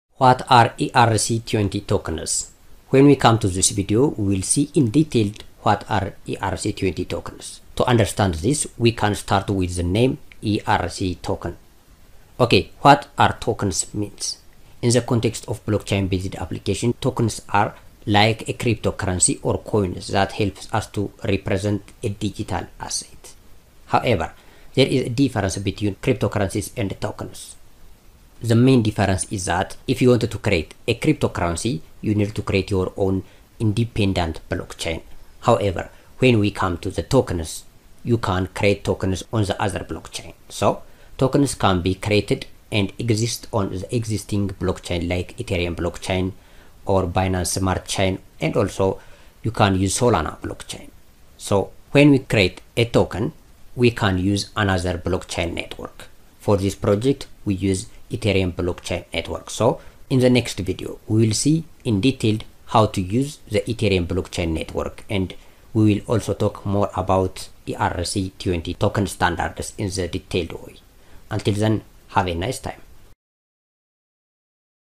ERC20 Tokens Explained: Unveiling Ethereum's Crypto Standard
Dive into the world of ERC20 tokens and unlock the secrets behind Ethereum's most fundamental standard. From defining ERC20 to exploring its significance in the crypto space, this video is your guide to understanding the backbone of Ethereum's token ecosystem. Whether you're a beginner or seasoned enthusiast, join us on this journey to unravel the mysteries of ERC20.